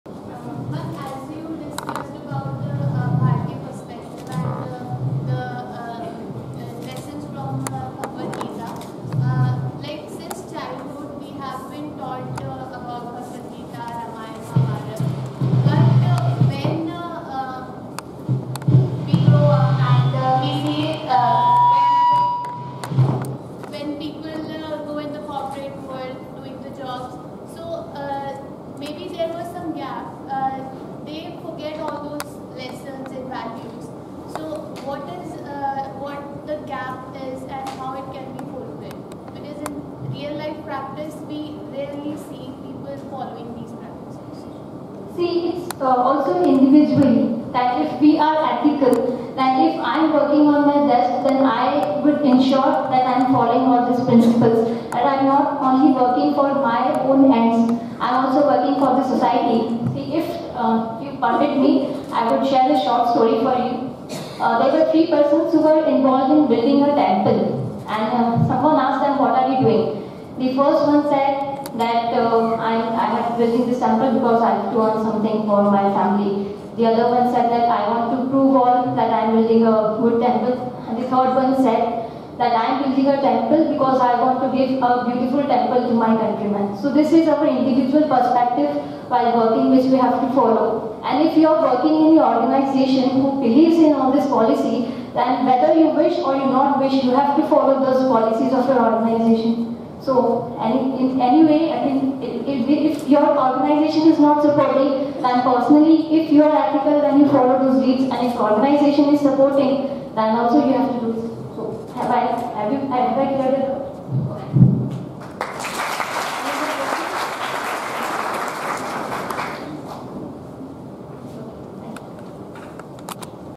Bharatiya Perspectives to CSR - Ms. Pallavi Joshi at Banasthali Vidyapith 2/2
Banasthali Vidyapith in association with National Foundation for Corporate Governance (NFCG) organized a National Conference on Corporate Governance (Leadership and Bharatiya Perspectives).